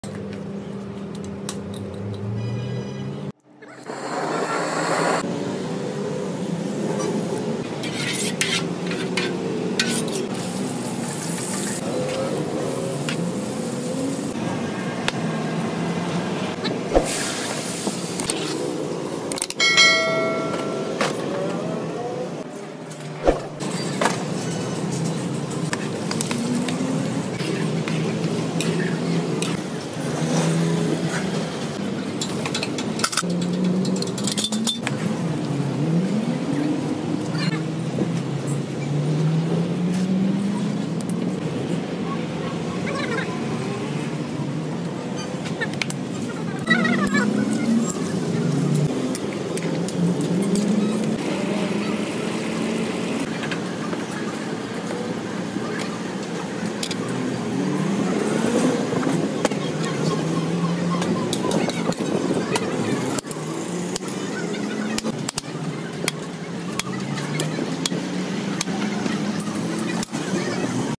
Kabuli Pulao Recipe 😋 Giant Meat Rice Prepared | Most Famous Afghani Pulao
Welcome to PeshawarZ channel
In this video you will see the full recipe of kabuli pulao and very interesting recipe.